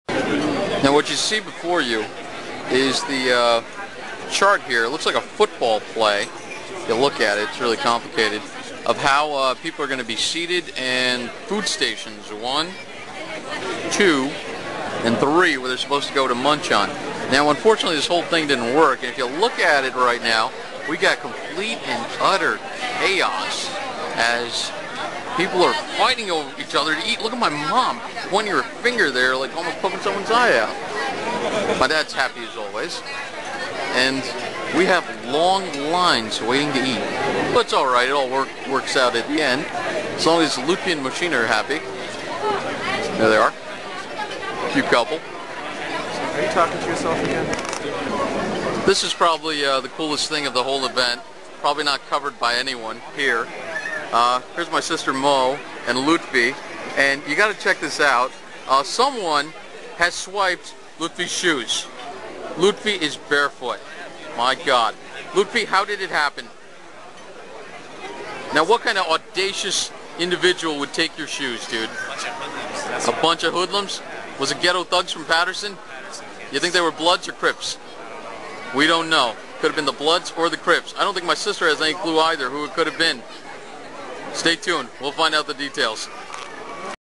Lutfi Loses His Shoes !!!
As Bengali wedding tradition goes, the bride's family must swipe the groom's shoes in order to blackmail the poor dude for some $$$. Being a brother of the bride, I felt it necessary to record this event...